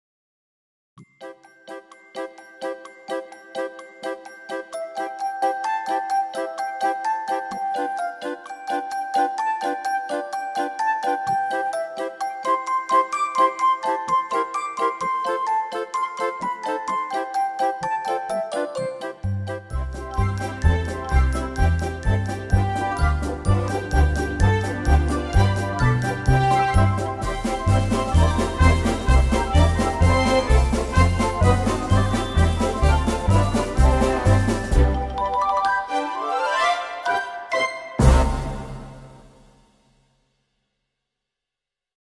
Sounds Like Series 8 - Disney - Showtime!
Complete brand new registrations. Sound like your favourite band or personality
5 exciting new software packs for Tyros-5. Each Sounds Like pack is made up of 5 banks each containing 8 Registrations. That is a total of 40 professionally made registrations with auto accompaniment.